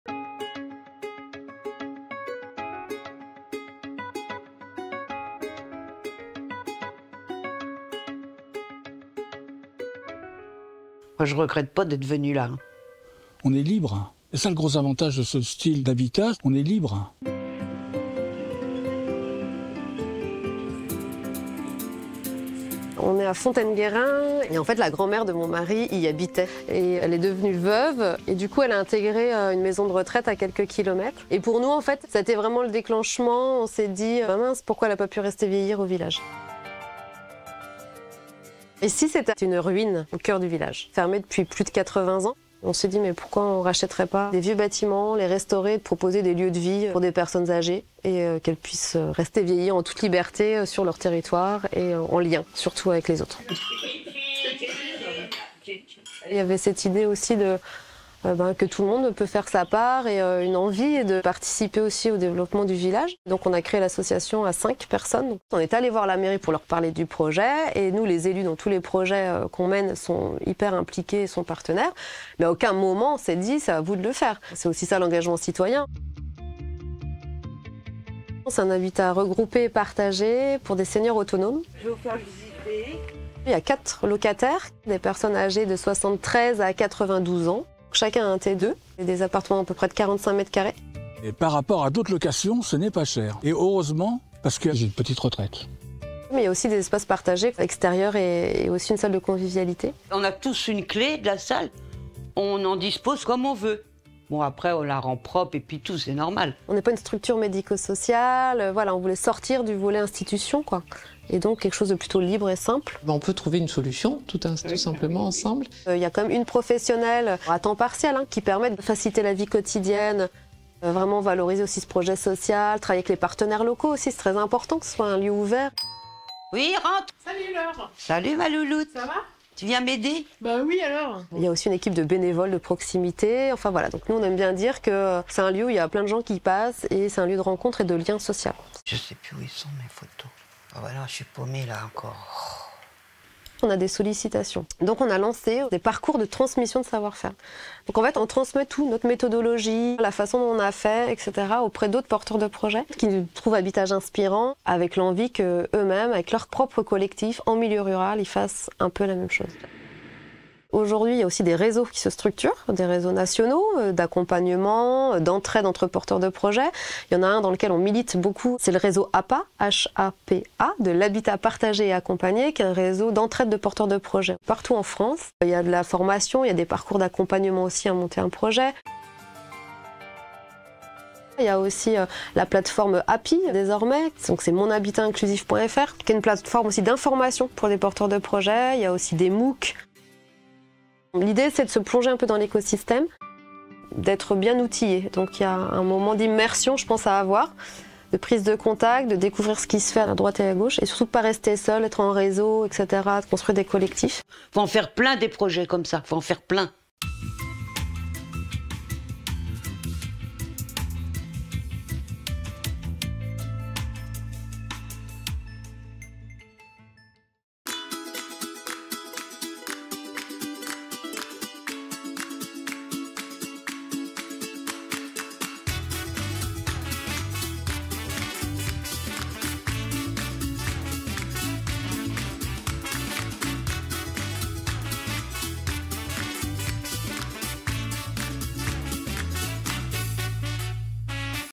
13 CRÉER UN HABITAT REGROUPÉ POUR SÉNIORS AUTONOMES SS T
L'association HABIT'ÂGE crée des habitats seniors solidaires au cœur des bourgs à partir de la restauration de patrimoine bâti et propose un accompagnement à la réflexion sur son "chez soi".
A l'initiative de deux aidants, un habitat partagé humain, participatif et solidaire de quatre habitations à vu le jour. Grâce à une salle partagée et à une coordinatrice dynamique et sympathique, les locataires vivent chez eux tout en étant impliqués dans la vie collective.
Partage d’expérience et bonnes adresses pour démarrer un projet de création d’habitat groupé.